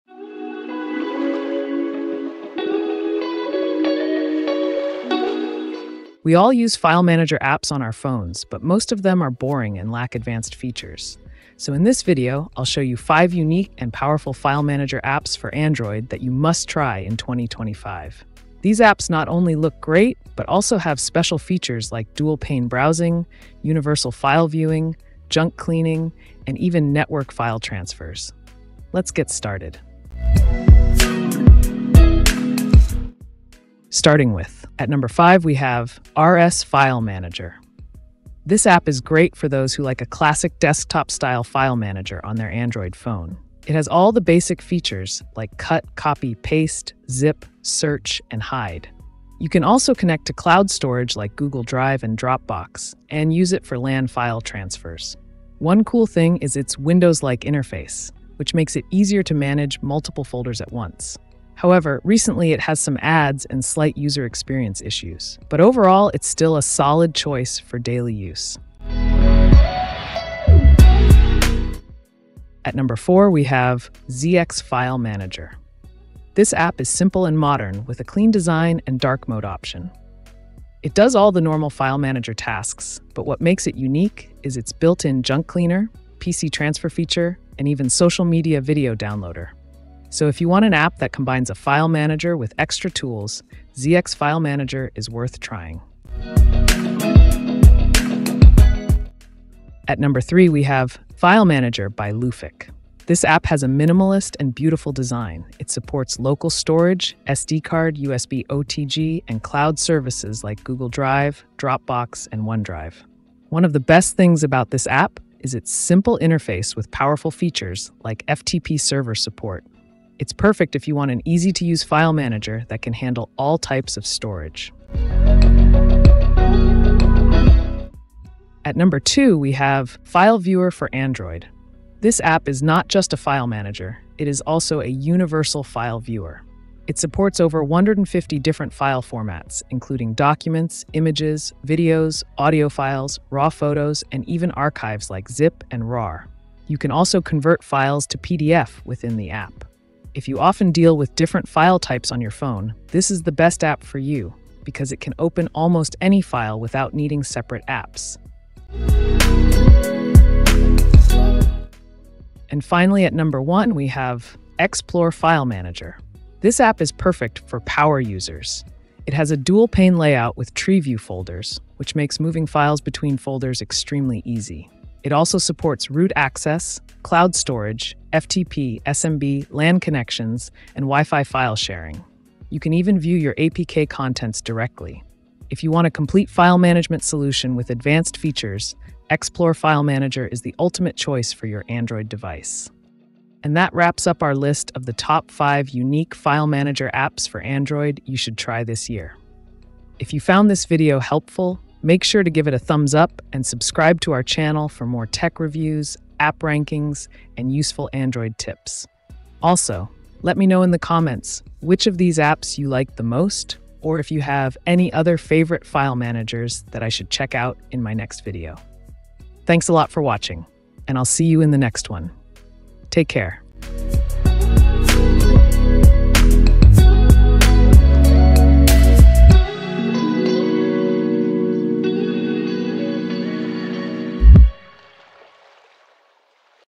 🔥 Top 5 Unique File Manager Apps for Android (2025) – You’ve Never Tried These!
**🎥 Video Title: Top 5 Unique File Manager Apps For Android You Must Try 2025**

Discover the **best file manager apps** that stand out from the crowd—perfect for everyday users and power users alike. In this video, we rank the top 5 most unique file managers of 2025 and explain what makes each one special. Watch, explore, and manage your files like a pro!

* ✅ **RS File Manager**: Classic desktop-style UI, cloud and network support, storage tools. Recent update adds ads—still powerful
* 🧹 **ZX File Manager**: Clean, dark mode, file operations + junk cleaner & social media downloader
* 🌿 **File Manager by Lufick**: Minimalist, cloud-integrated, FTP & USB‑OTG support
* 📂 **File Viewer for Android**: Universal viewer for 150+ file types, PDF converter, lightweight and efficient
* 🔧 **X‑plore File Manager**: Dual-pane explorer, root/LAN/FTP/SFTP, cloud & Wi‑Fi sharing — ultimate power user app

Learn why each app is unique, their key features, and which one suits your workflow best.

* ✅ **In-depth Analysis**: We break down each app’s interface, features & special strengths
* ⚡ **Smart Comparisons**: Helps you move beyond mainstream apps to find what fits
* 📈 **Future-Proof Picks**: These 5 apps are the best-of-breed for 2025
* 🎬 **Time-Saving Timestamps**: Jump right to the apps you're curious about

## 🎯 Timestamps

00:00 – Intro
00:33 – #5 RS File Manager
01:12 – #4 ZX File Manager
01:39 – #3 File Manager by Lufick
02:08 – #2 File Viewer for Android
02:42 – #1 X-plore File Manager
03:13 – Outro + Recommendations

## 📥 Download These Apps

* **RS File Manager** – Free, desktop-style file explorer with cloud & network support

* **ZX File Manager** – Sleek, dark-mode file manager with junk cleaner & social media downloader

* **File Manager by Lufick** – Minimalist UI, cloud access, SD/USB‑OTG support

* **File Viewer for Android** – Opens 150+ file types, built-in PDF converter

* **X‑plore File Manager** – Dual‑pane explorer with root, LAN/FTP, cloud & Wi‑Fi support

Drop a comment to share your pick or suggest other hidden gem file managers 🧠